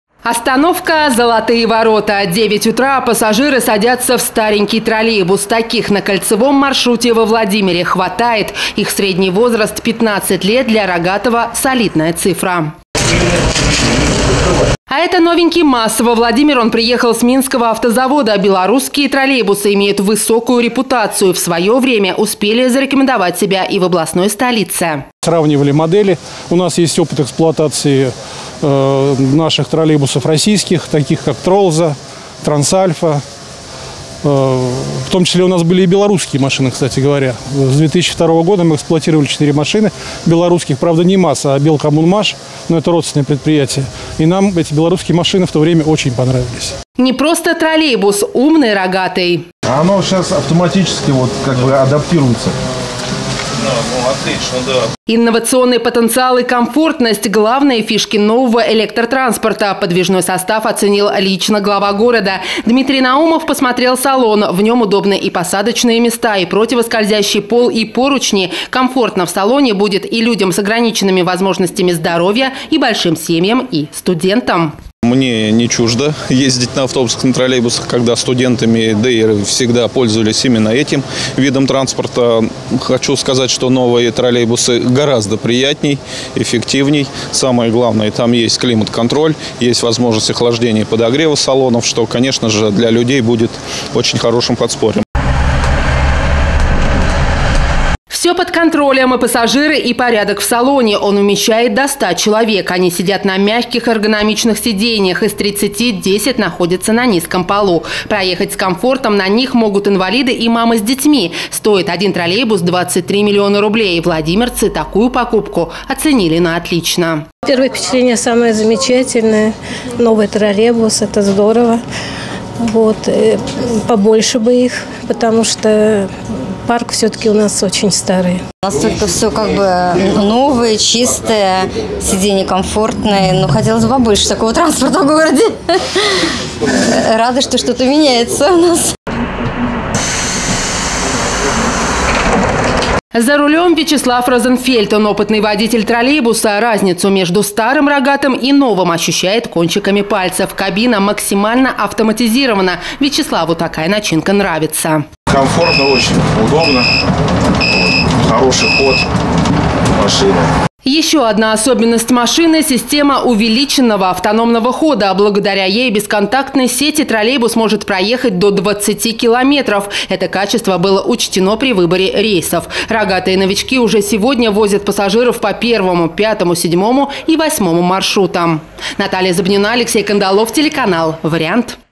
Во Владимире на линию вышли 4 новых троллейбуса с Минского автозавода
Каждый "рогатый" вмещает сто пассажиров, оборудован системой кондиционирования салона и электронными табло